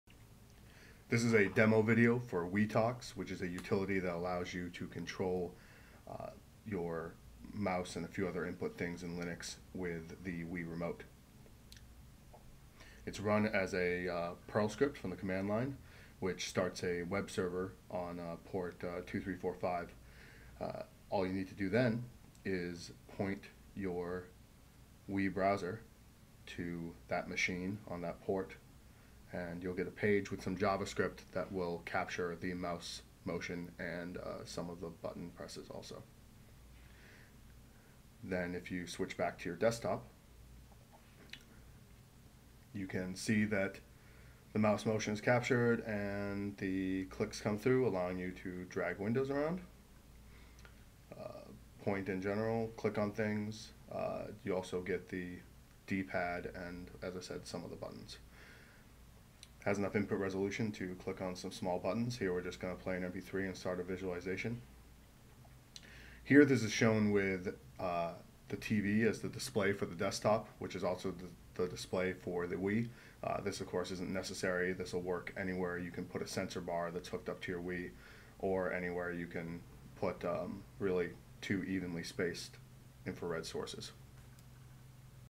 wiiToX demo
wiitox allows you to use a wii remote to control your mouse in linux.